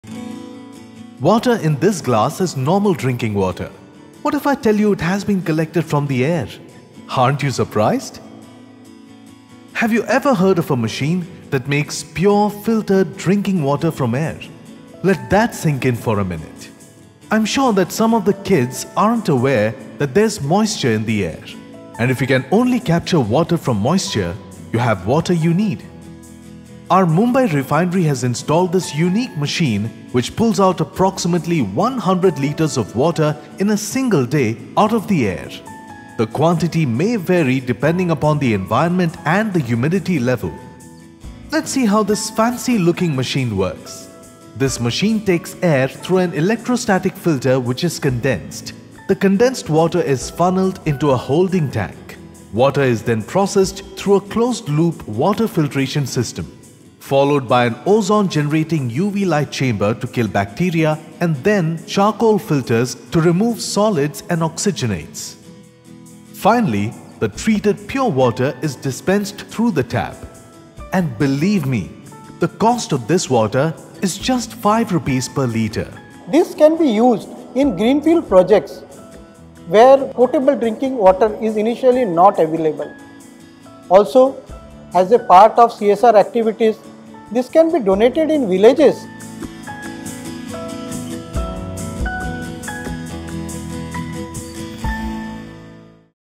How to get water from air
Talk about pulling things out of thin air. This is exactly what this unique machine at our Mumbai refinery does. It takes moisture from the air and converts it into water that is safe for human consumption. At Rs. 5 per litre, this water maker could be a boon to human kind, at places where drinking water is not available.
Keep watching to know how it pulls water out of thin air.